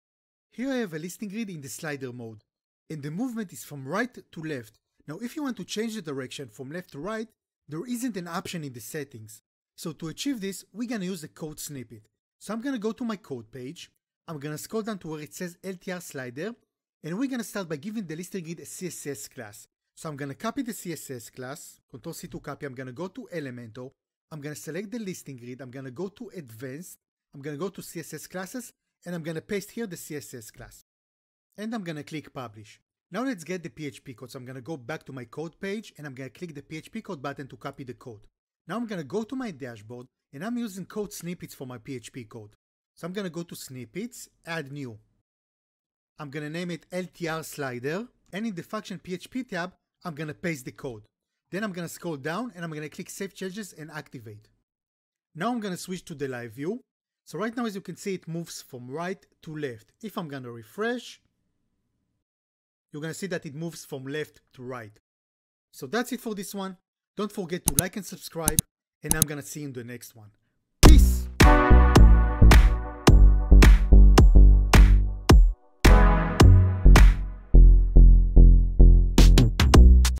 Change Slider Direction | jetengine
Explore the easy steps to transform your grid listing slider's movement in this tutorial! If you've ever wanted to change the direction from right to left or left to right but found no option in the settings, we've got you covered.